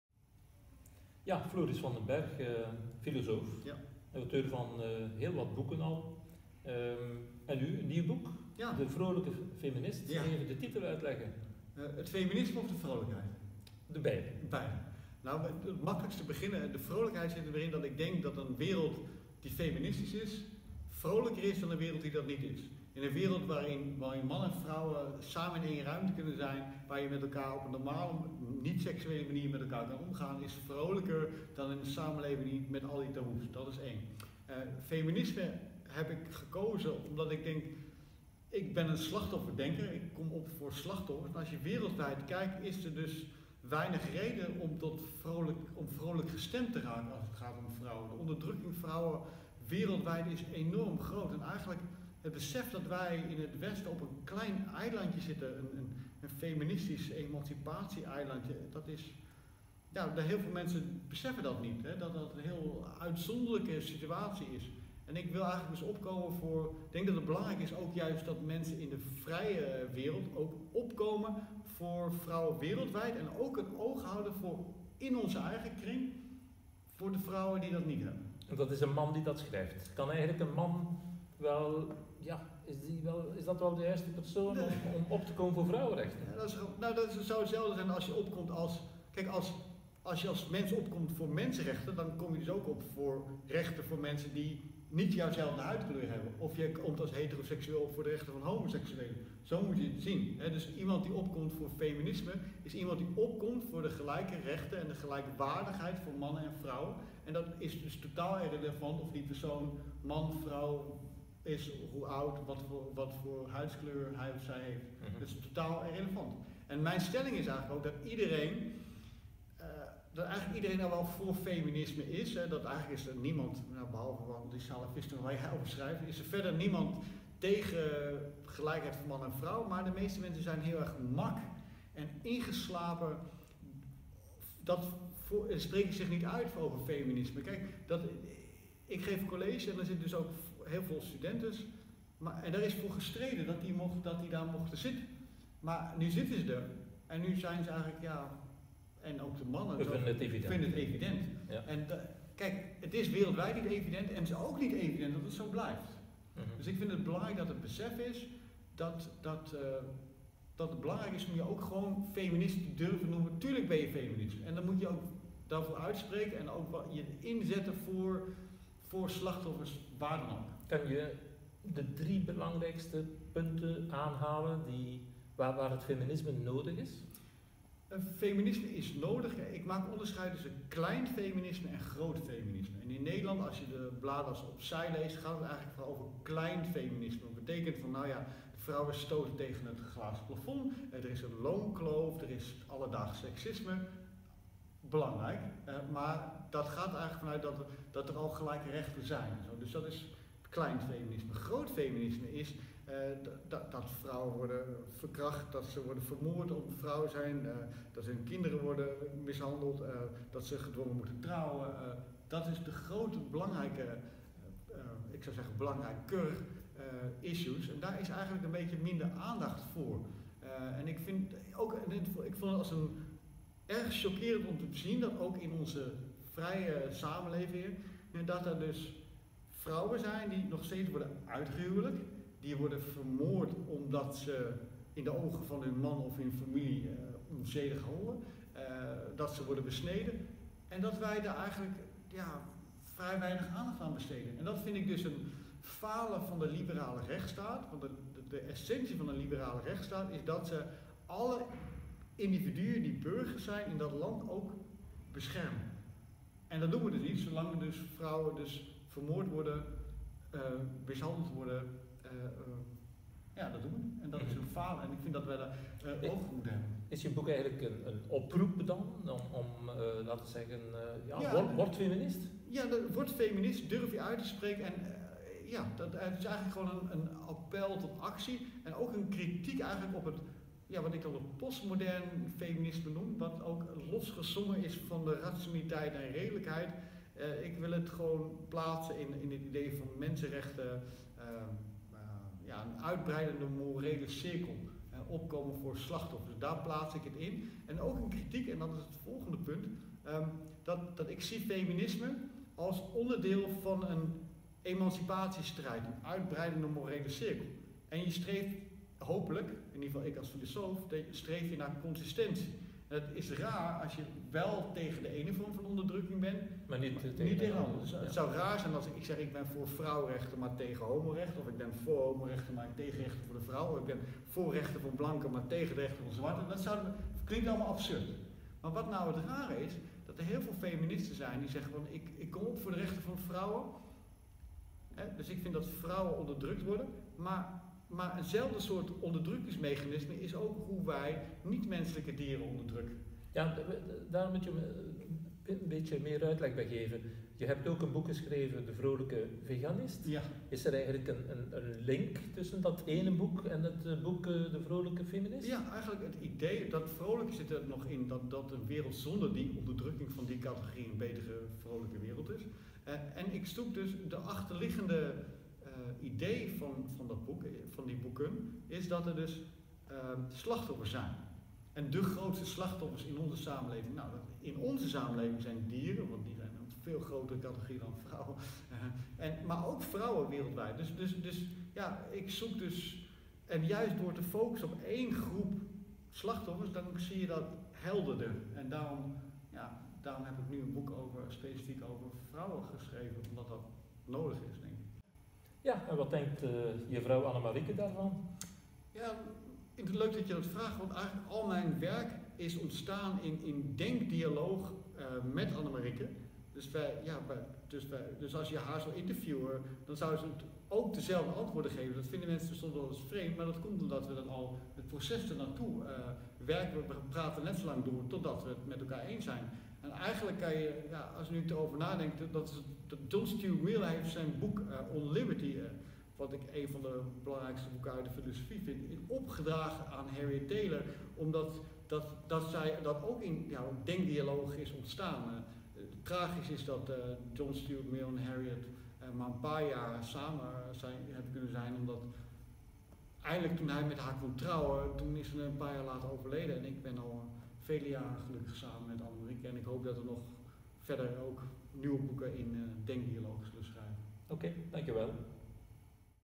De vrolijke feminist
Dirk Verhofstadt interviewt Floris van den Berg